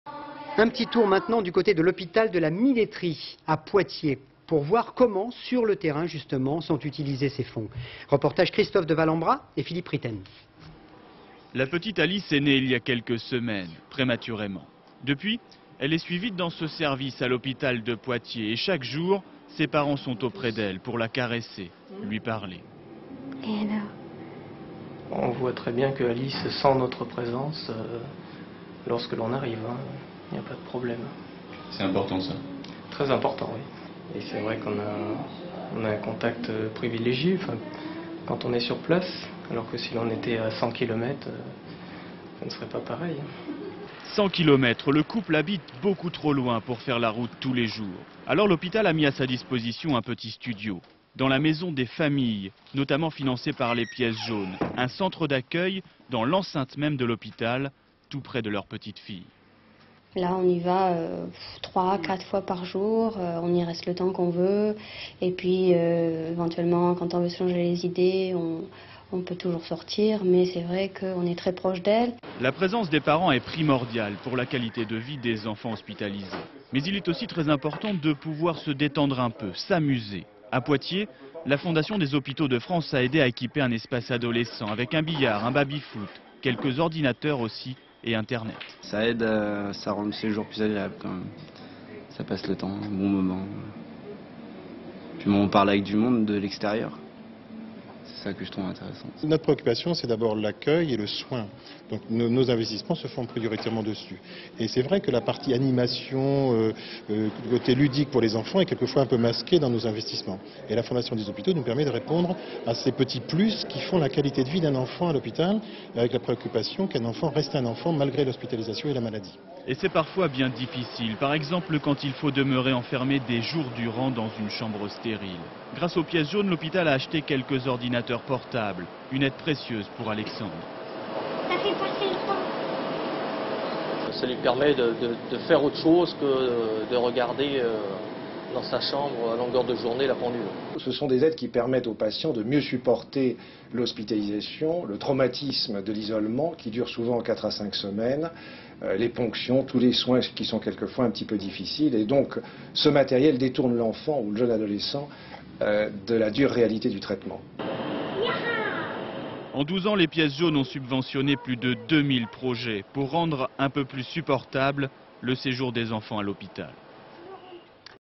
Opération pièces jaunes
8 janvier 2001
Reportage à  l'hôpital de La Milétrie à Poitiers, où un jeune couple Images d'archive INA